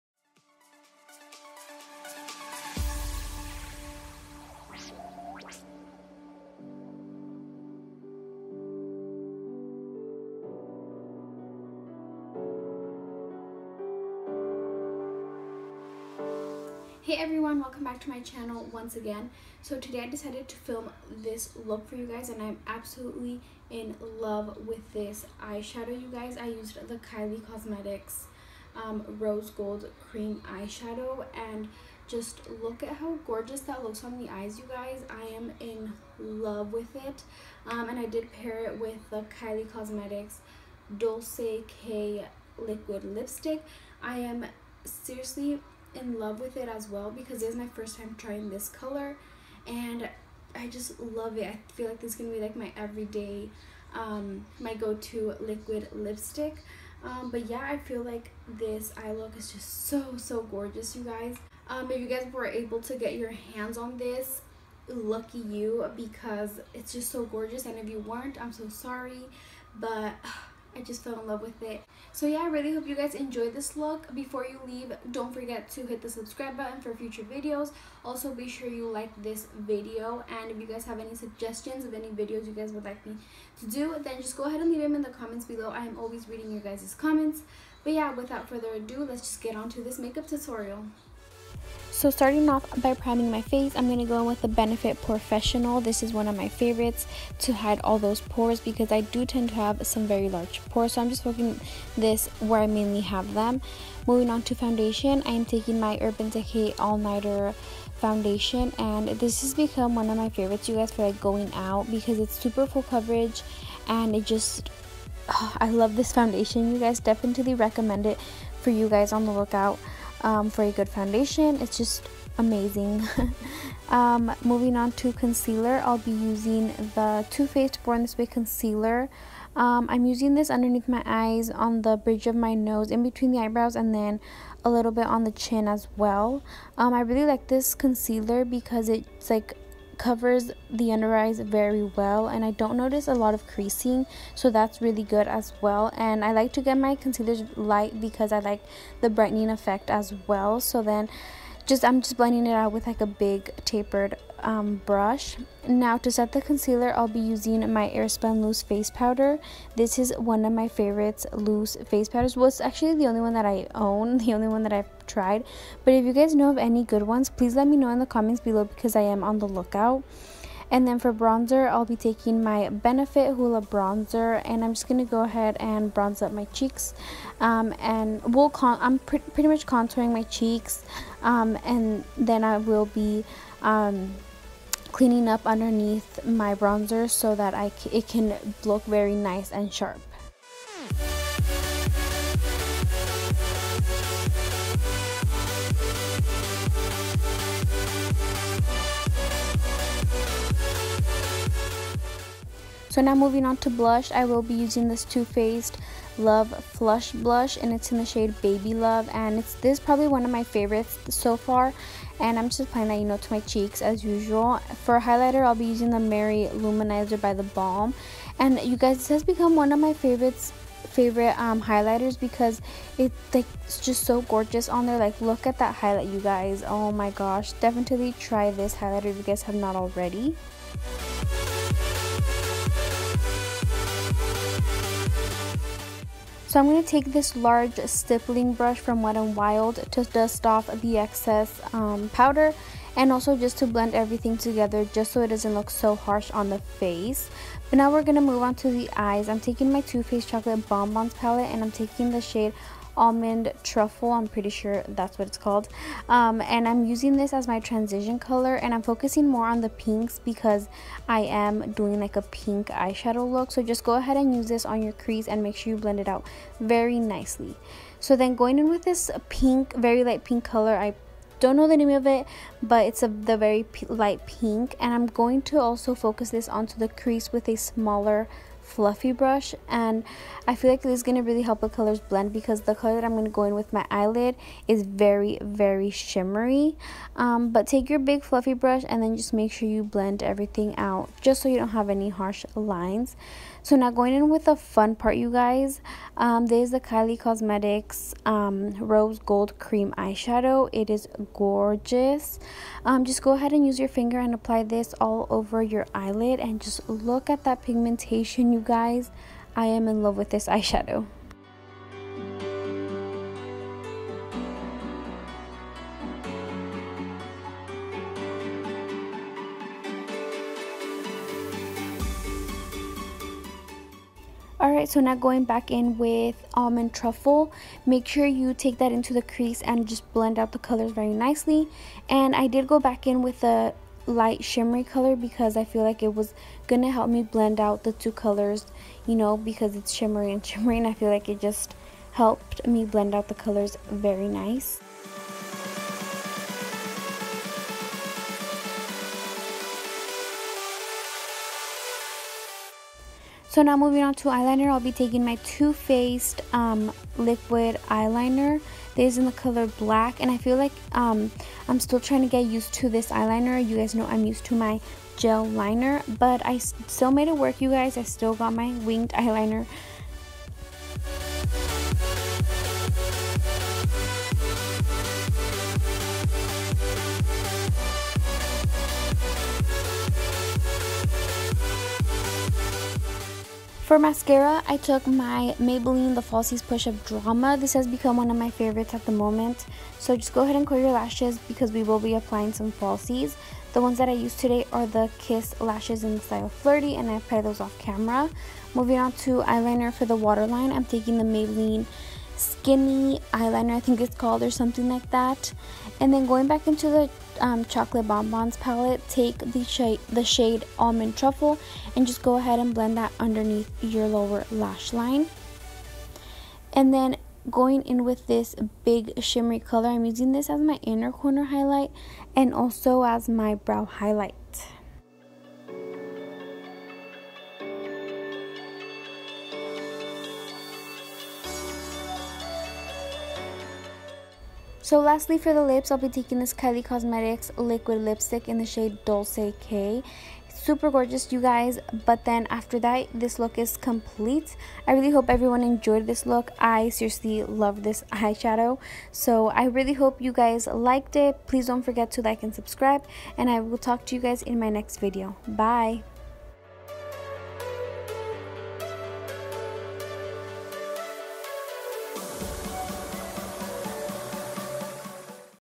Rose Gold Eyes & Matte Lips | Kylie Cosmetics | Adriana Casarez
Welcome everyone :) Today I filmed a tutorial using Kylie cosmetics creme eyeshadow and liquid lipstick.. I fell in love with the creme eyeshadow. This look is super simple but so glamorous on the eye. I hope you enjoy it

💋PRODUCTS MENTIONED:
Benefit porefessional
Urban decay all nighter foundation (4.5)
Too faced blush (baby love)
The balm marylouminizer
Too faced chocolate bon bon palette
Kylie cosmetics rose gold creme shadow
Too faced liquid eye art pen
Maybelline push up drama mascara
Kiss lashes (flirty)
Kylie cosmetics liquid lipstick (Dolce K)